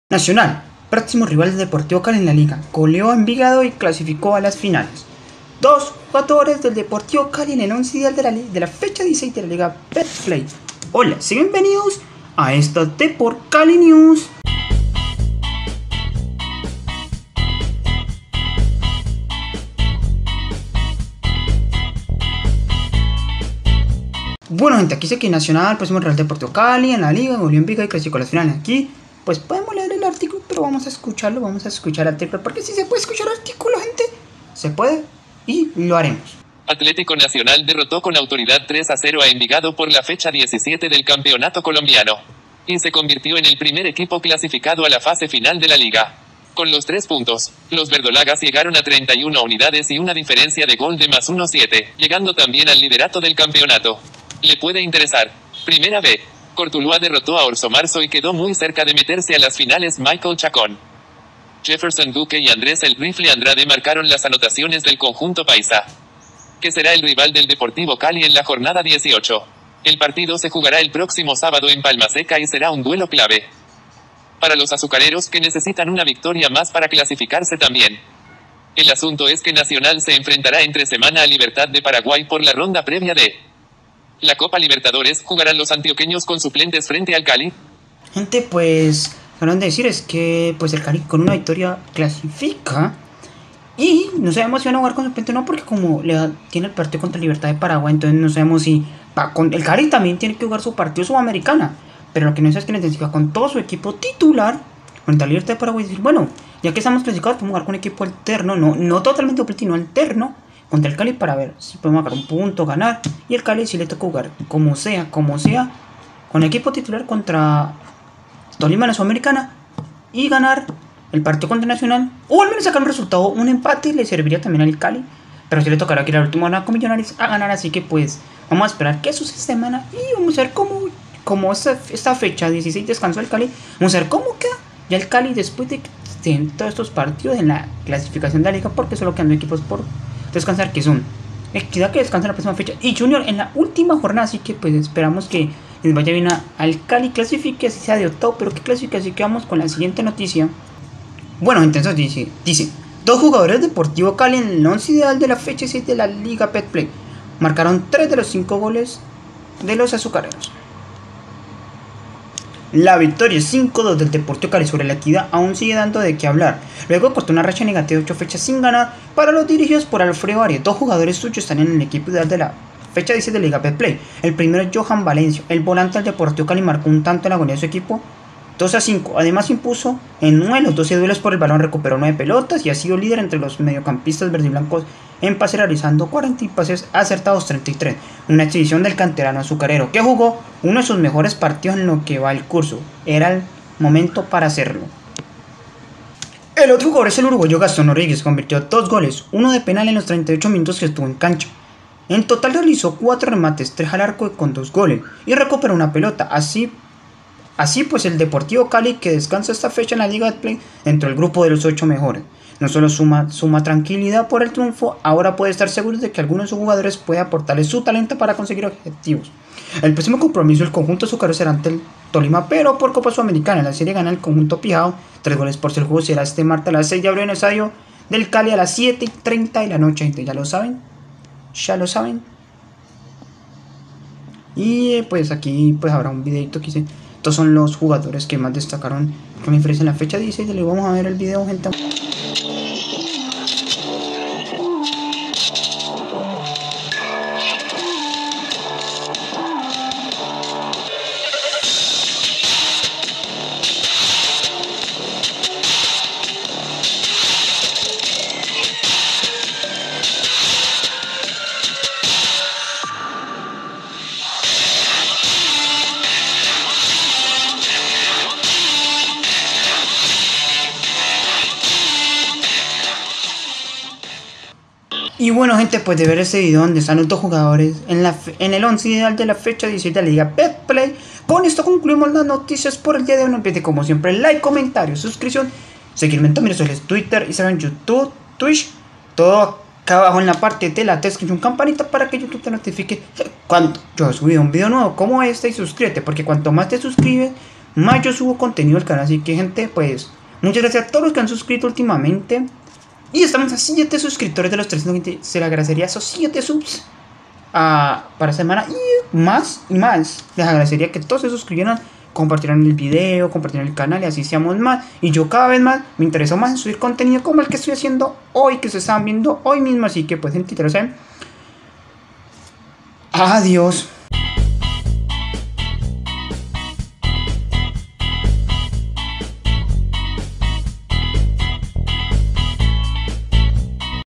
EL CALI VISTA AL NACIONAL POR LA FECHA 17, EL CALI TIENE 2 JUGADORES EN EL 11 IDEAL DE LA FECHA 16.
hola en este te cuento que el cali ya esta preparando el partido contra el nacional por la fecha 17, hay dos jugadores del cali en el 11 ideal de la fecha 16, esto y mucho mas en las depor cali news.